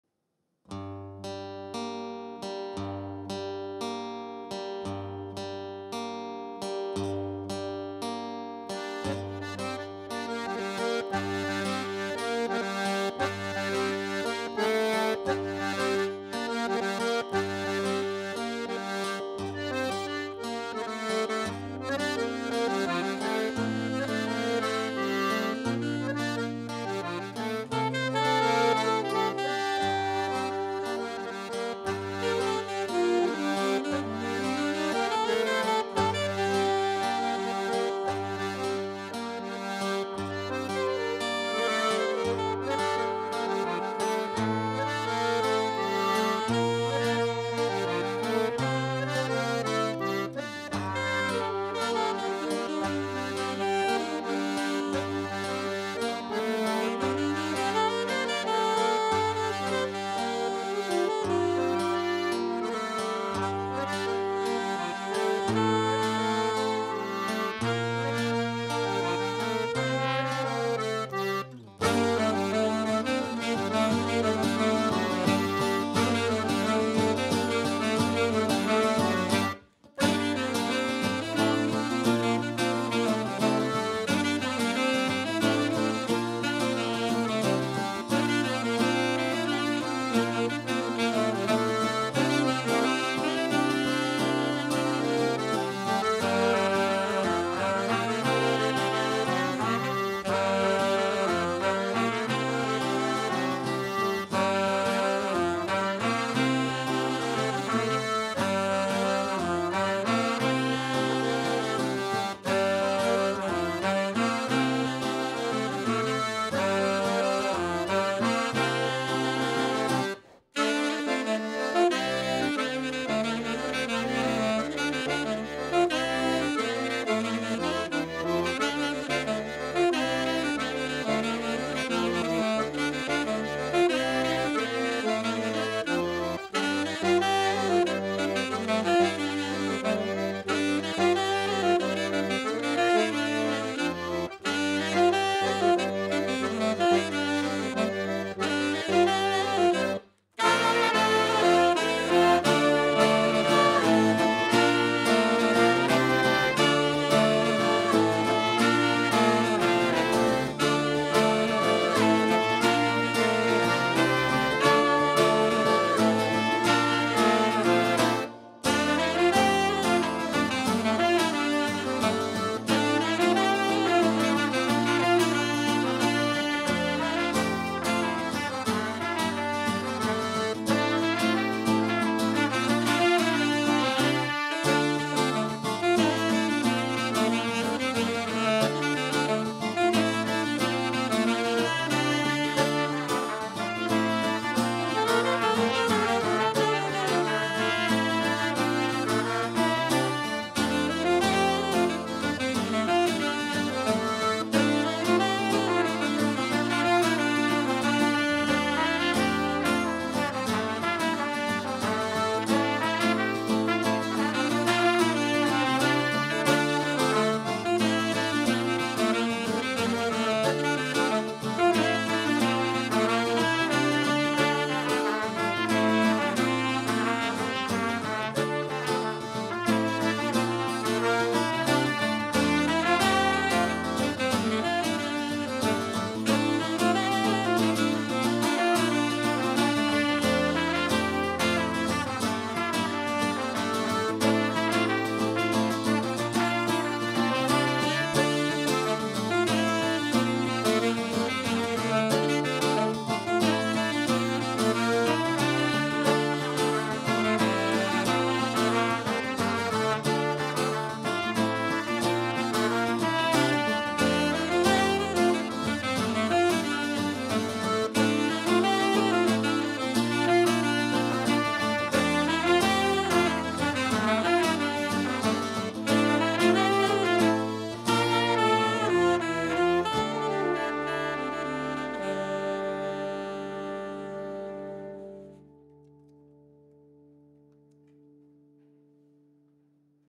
Ar strollad Llym e-barzh Bali Breizh : Hanter-dro
Da vare e droiad e Gwengamp e oa bet skipailh Bali Breizh o kejañ gant ur strollad sonerezh paotred yaouank a Vro-Dreger ha leun a startijenn hag a ijin : Llym.
Setu un hanter-dro.

Lors de sa balade à Guingamp, l'équipe de Bali Breizh est allée à la rencontre d'un groupe de musique composé de jeunes artistes trégorrois
plein d'énergie et d'imagination : Llym.
Voici un hanter-dro.